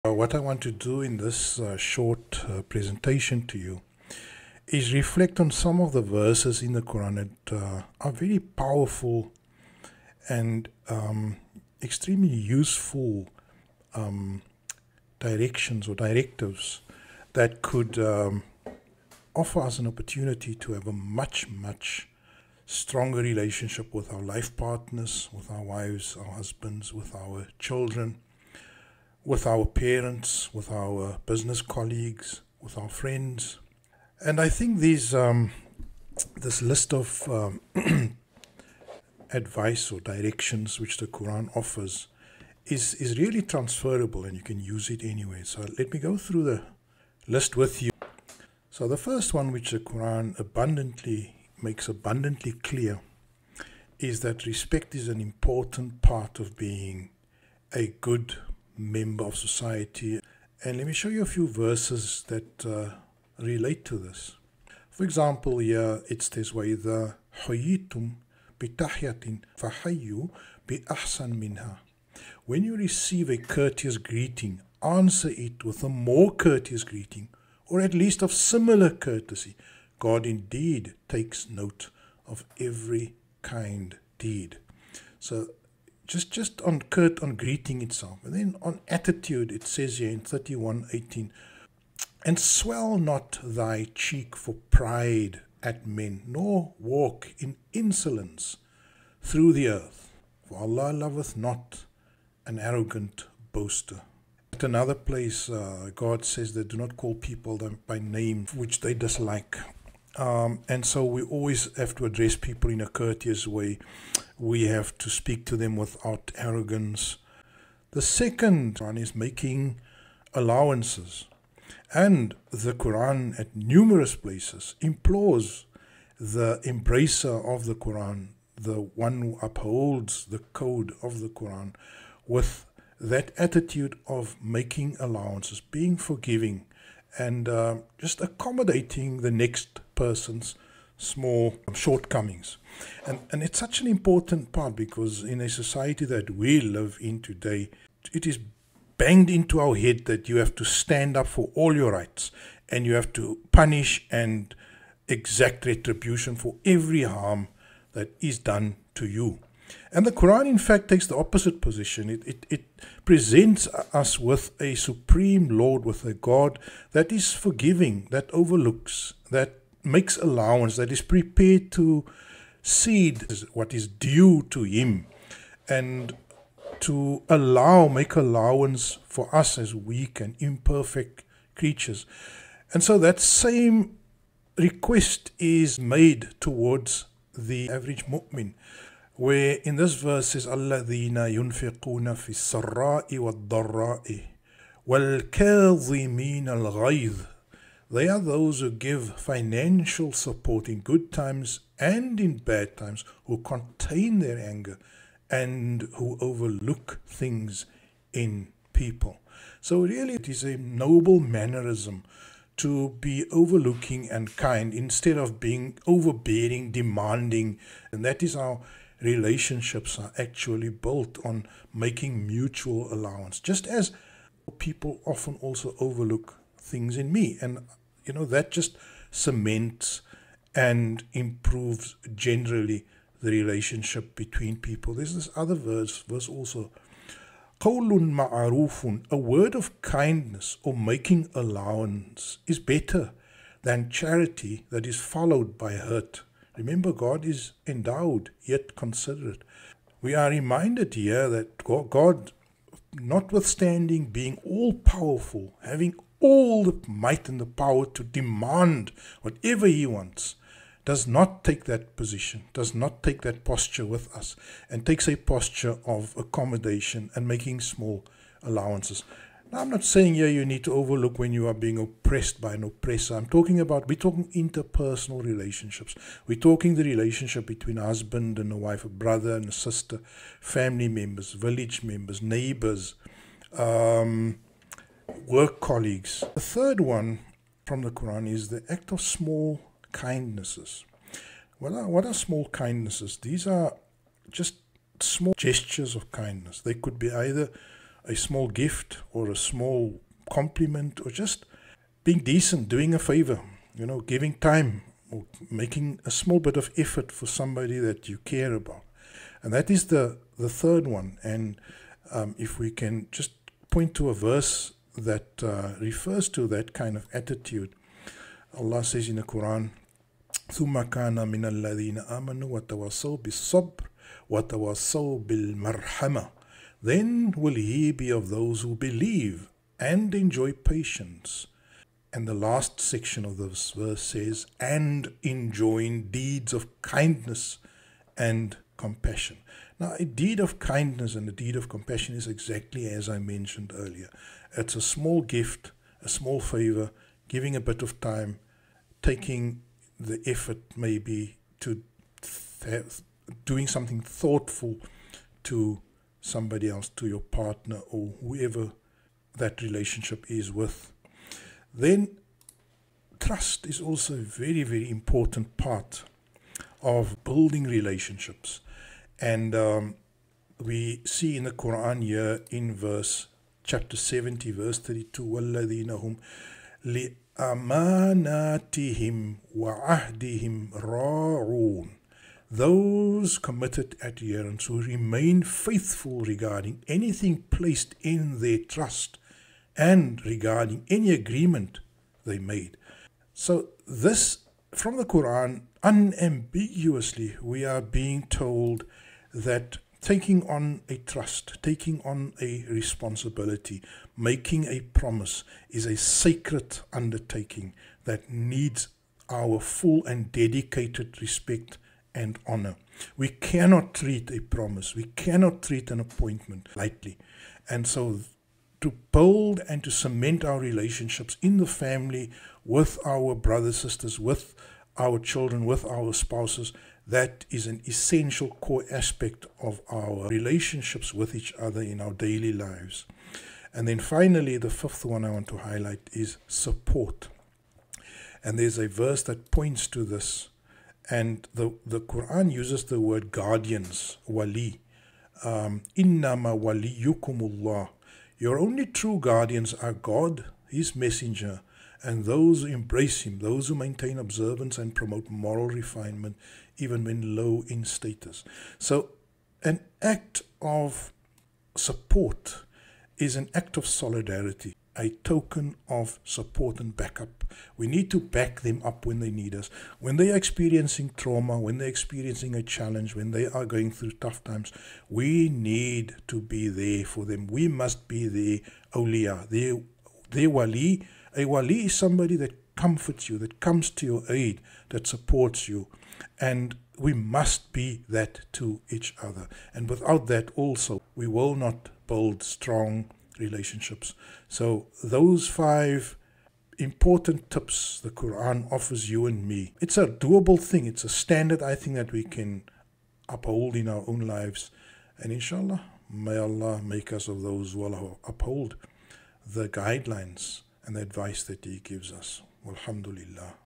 5 Ways in which to improve your relationships with people around you.
The Quran offers these useful everyday tips for us to maintain healthy and productive relationships with those around us.